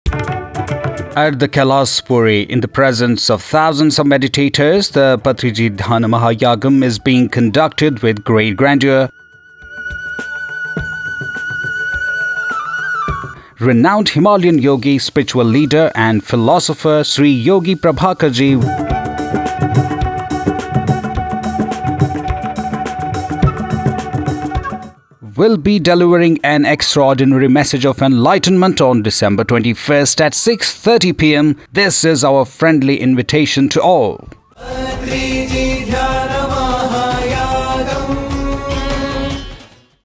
Shri Yogi Prabhakar Ji | Gurus | Patriji Dhyana Mahayagam 2023 | PMC English
"Be A Master and learn from every Master." - Brahmarshi Pitamaha Patriji

All are cordially invited to the event of the year Patriji Dhyan Mahayagam.
Join us on 21st December for a message from Guru Shri Yogi Prabhakar Ji
Venue: Maheshwar Maha Pyramid, Kailashpuri, Kadthal, Hyderabad.